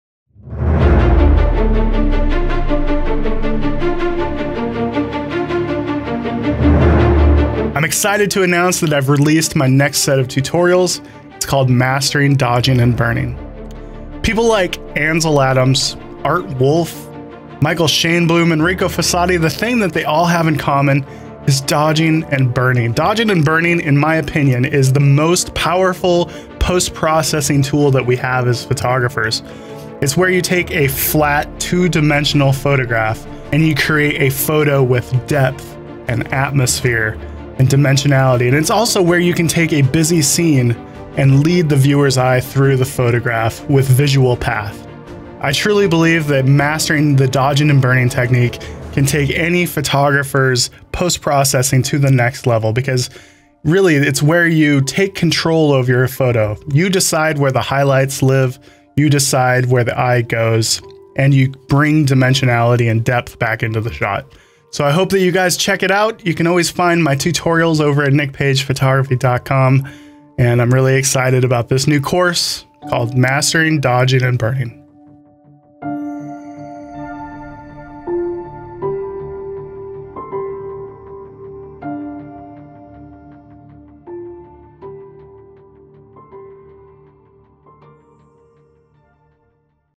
Introducing - Mastering Dodging and Burning
I am excited to announce the release of Mastering Dodging and Burning.

In these videos Nick shows you the most powerful ways to dodge and burn

Covered in this course:

Why Dodging and burning is such a crucial part of the editing process

Freehand dodging and burning

Guided Dodging and burning

Dodging and Burning using smart objects

Dodging and Burning using contrast adjustments

Creating Depth and atmosphere

Creating visual path

Creating Color separation

This is a 11 video set, with a Combined runtime of around 1.75 hours

 *note, this is a photoshop tutorial.  All post processing will be taken place inside of photoshop and adobe camera raw.  It is highly encouraged that you have a basic understanding of layer masks, and a general idea of the concept of luminosity masks.